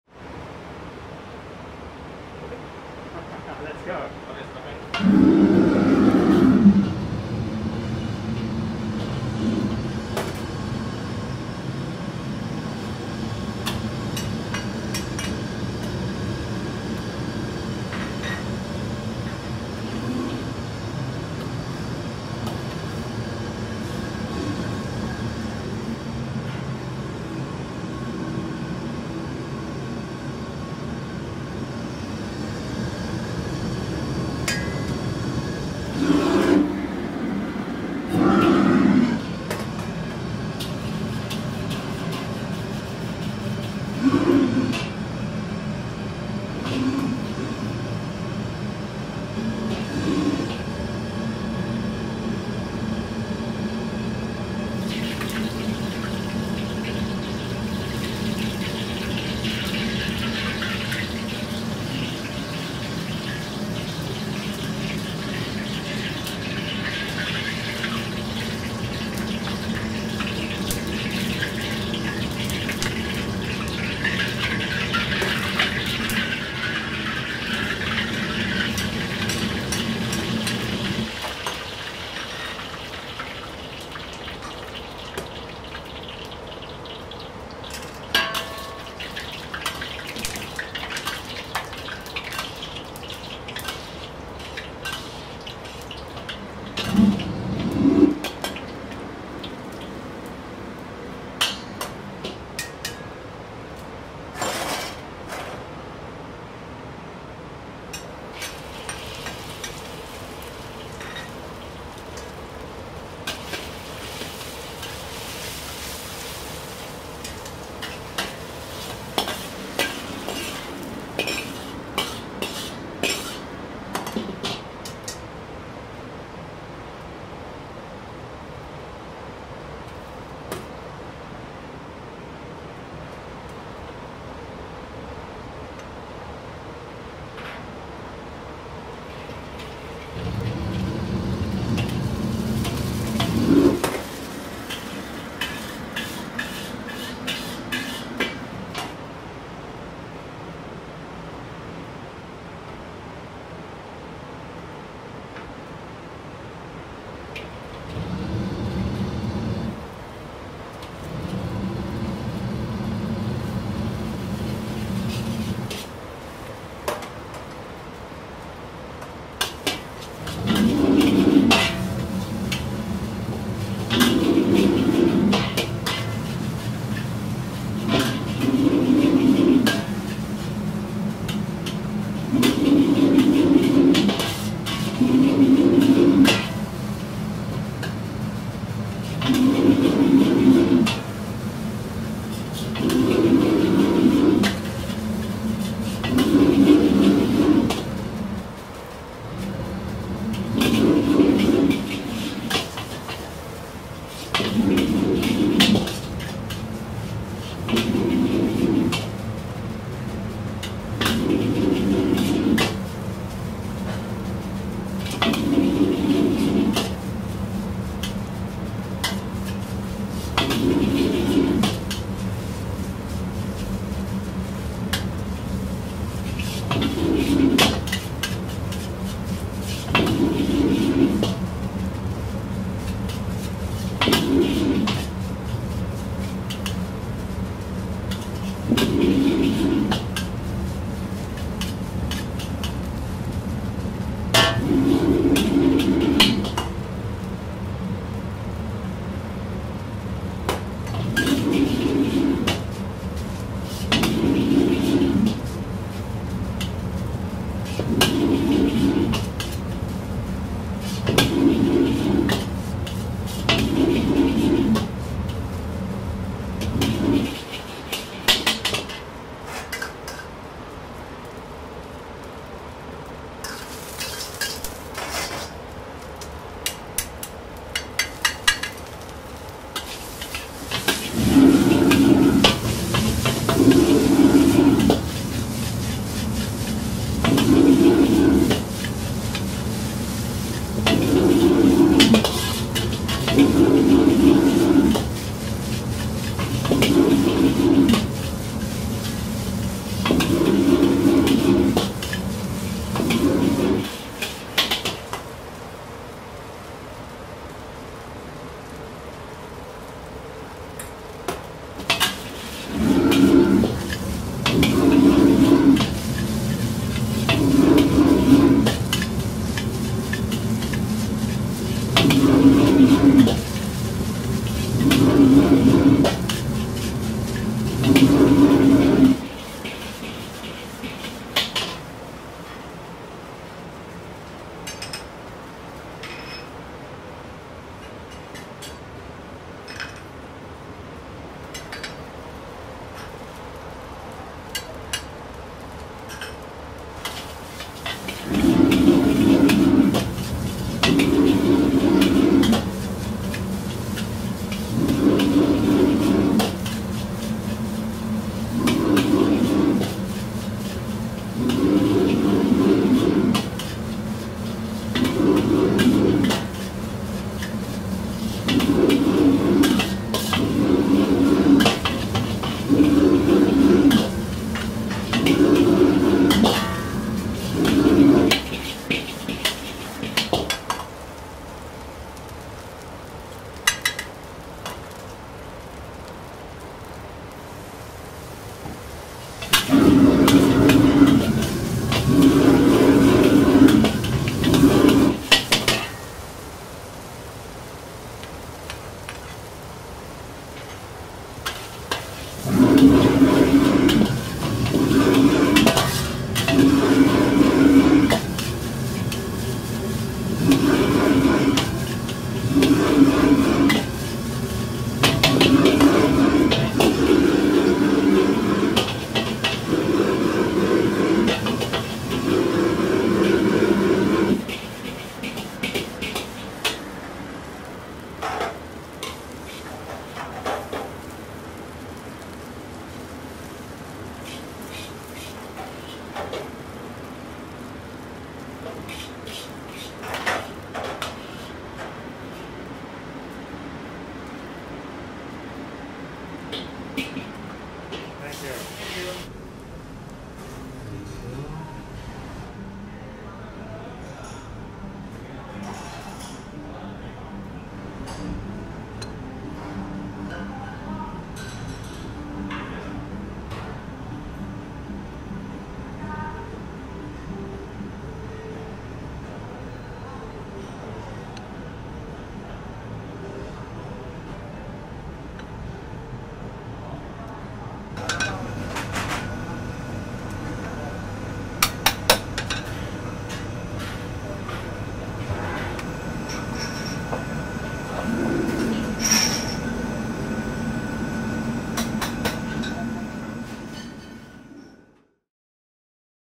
$46 High-end Fried Rice - Hong Kong
Fried Rice at Ming Court Wanchai - Ingredients: Foie Gras, Wagyu Beef, Spring Onion, Purple Onion, Chinese Celery, Beans, Egg White, Egg Yolk, Fried Garlic, Rice, Secret Barbecue Sauce